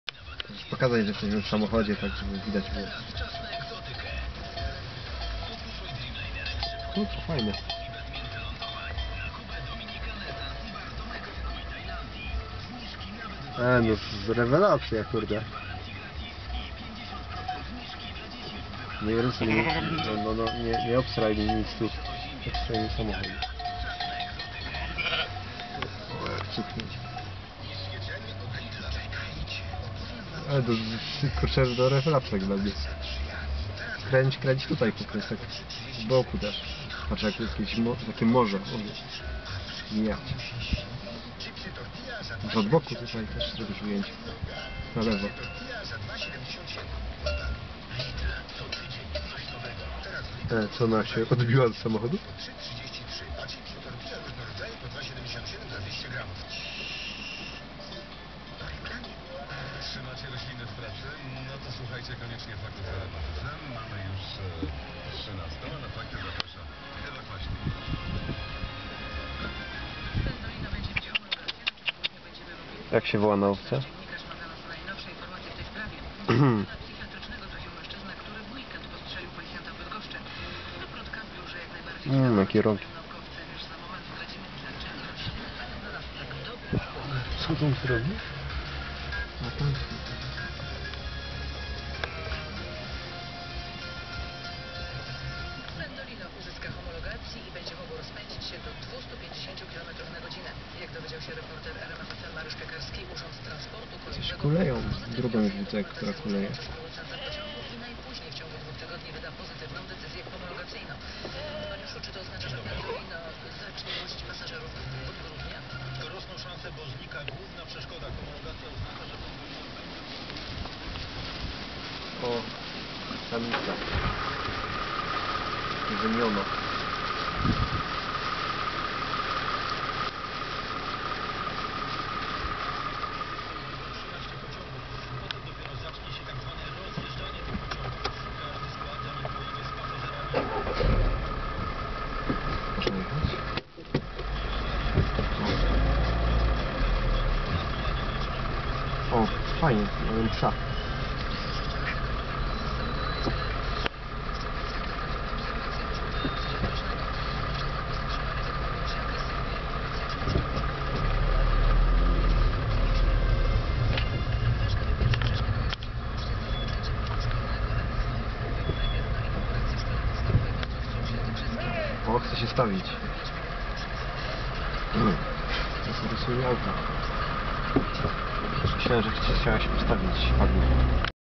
A sheep herd on a Polish road between Tylawa and Cisna
On the 1st of September, 2014 our journey to the Bieszczady mountains was shortly interrupted by a sheep herd we had encountered on a section of a regional road between Tylawa and Cisna in the South-Eastern Poland.